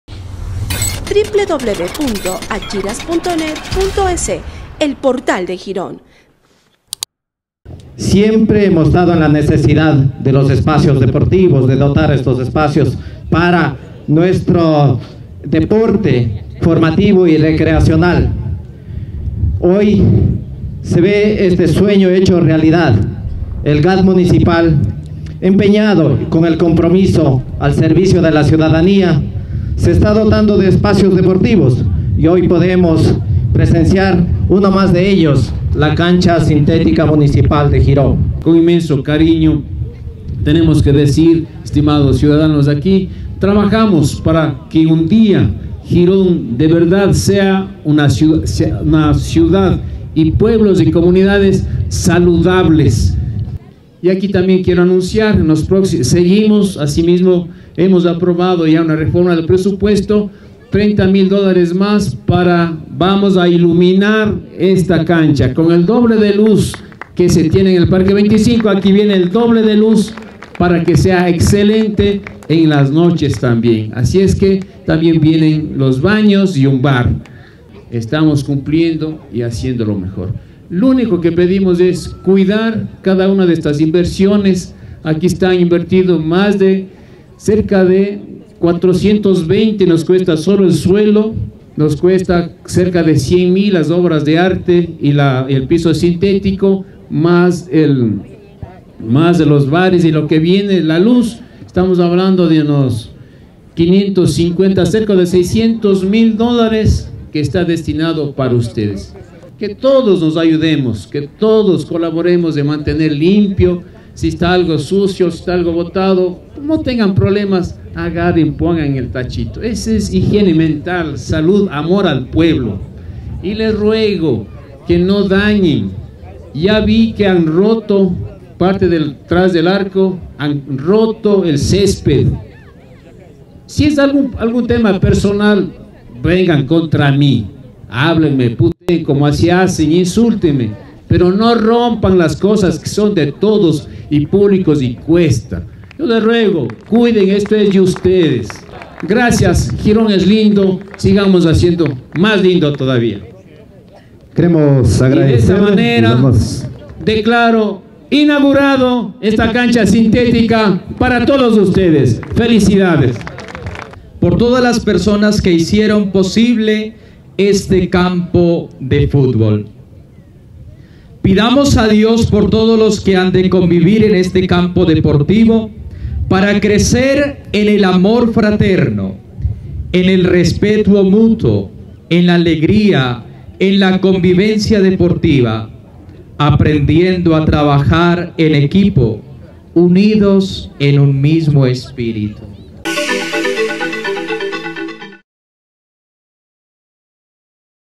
Cancha municipal con piso sintético entró en funcionamiento.
La cancha con piso sintético de 80 x 40 metros para futbol 9, construida por administración directa de la administración municipal sobre un terreno adquirido recientemente junto a predios de LDCG, entró a funcionar luego de que en la mañana del domingo 23 de septiembre, en un acto público se desarrolló la inauguración y bendición de las instalaciones.
Es la primera etapa que esta concluida y faltan otras obras por complementar dio a conocer el alcalde José Miguel Uzhca. Acto seguido se desarrollaron encuentros.